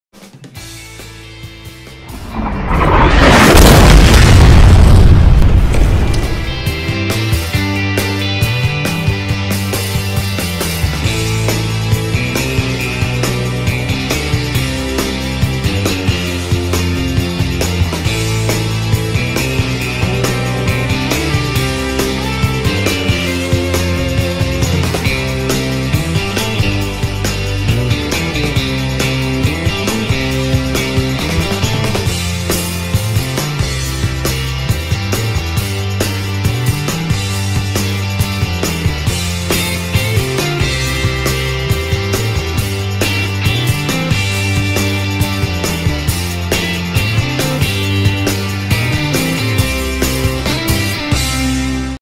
Short Course Body for Slash
We have recently spent some time painting a few bodies.  This video shows a few different stages in painting a short course body for a Traxxas Slash.